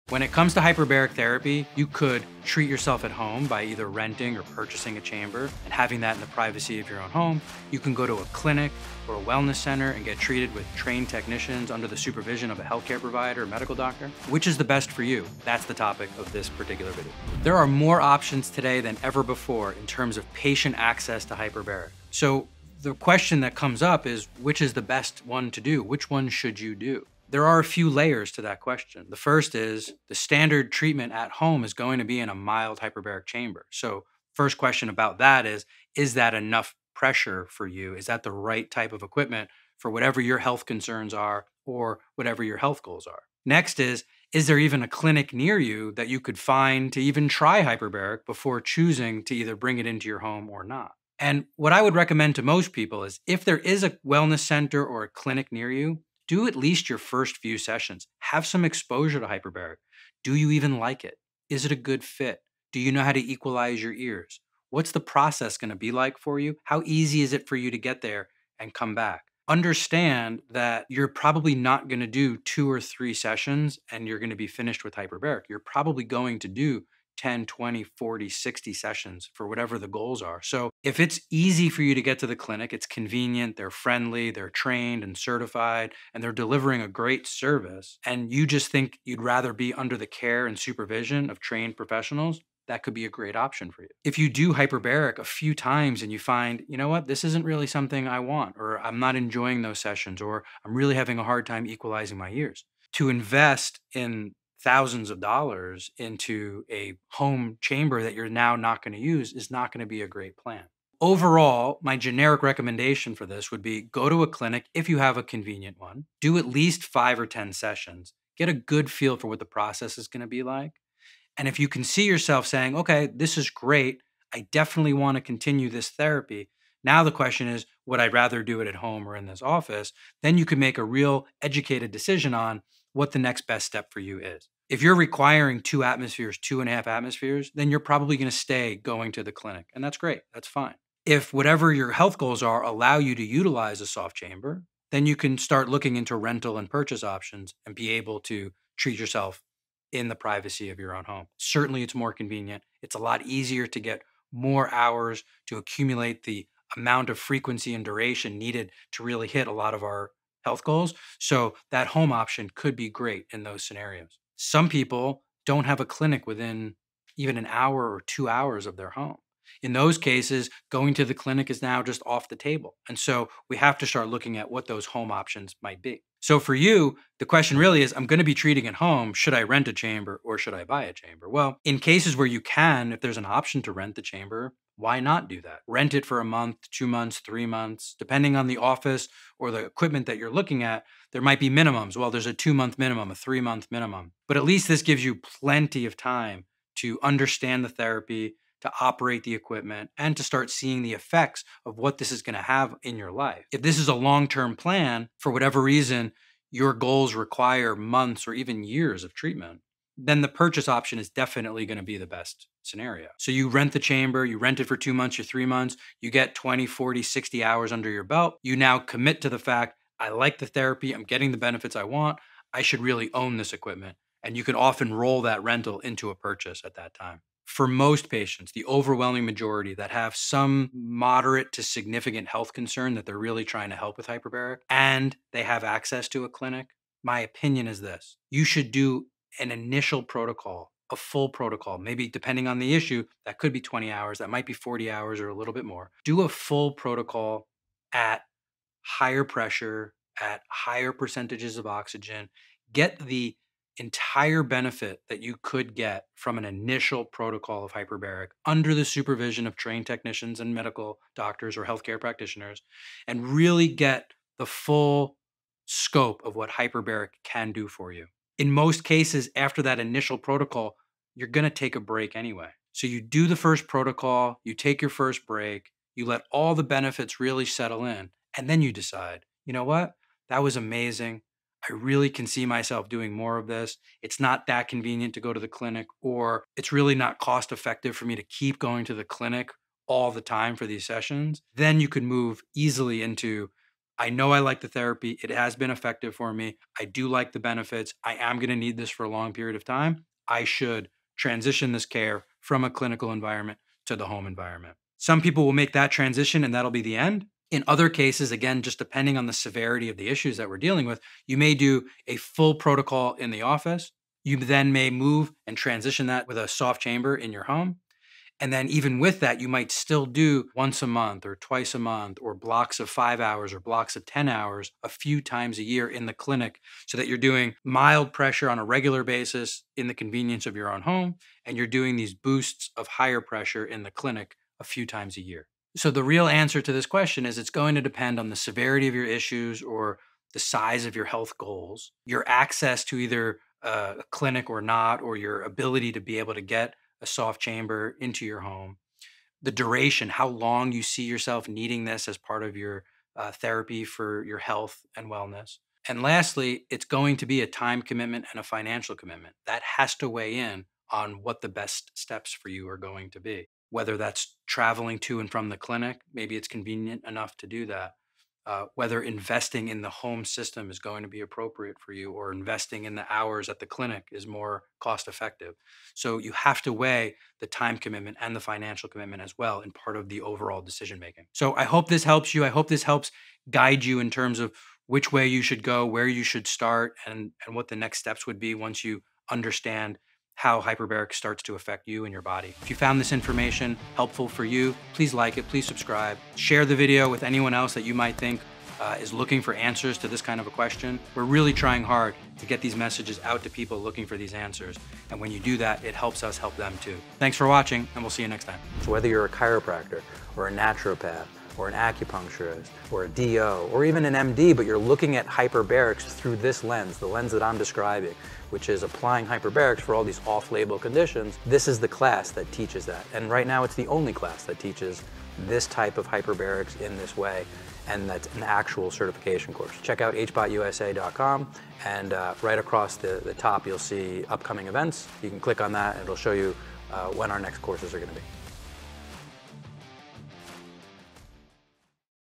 Can You Do Hyperbaric Oxygen Therapy At Home?
Watch this video to learn when to do HBOT at home vs. in a clinic.

Should you rent or buy a hyperbaric chamber and treat yourself at home, or should you go to a clinic that offers hyperbaric oxygen therapy?

Which is the best for you?

Overall my generic recommendation would be go to a clinic if you can and do at least five or ten sessions. Get a good feel for what the process is going to be like and see if you like the therapy.

If so, then you can make a real educated decision on what the next best step for you is.

Note: If you're requiring more than 1.3 atmospheres due to your health issues or goals, then you'll need to keep going to the clinic.

But if your health goals allow you to utilize a soft chamber, then you can start looking into rental and purchase options and be able to treat yourself in the privacy of your own home.